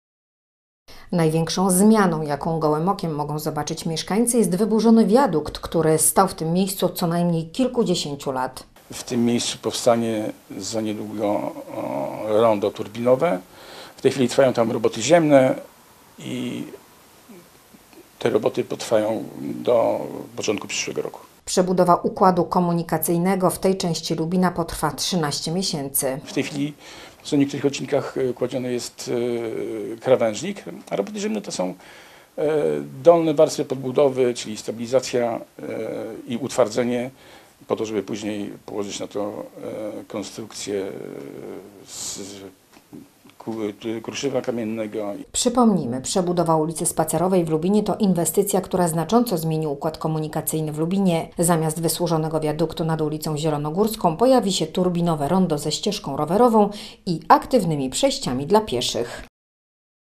LUBIN. Kolejny etap remontu Spacerowej
Remont ul. Spacerowej w Lubinie wchodzi w kolejną fazę. Wyburzono już wiadukt. Prace wykonuje wrocławska spółka Pro-Tra Building, która ma 13 miesięcy na wykonanie wszystkich prac.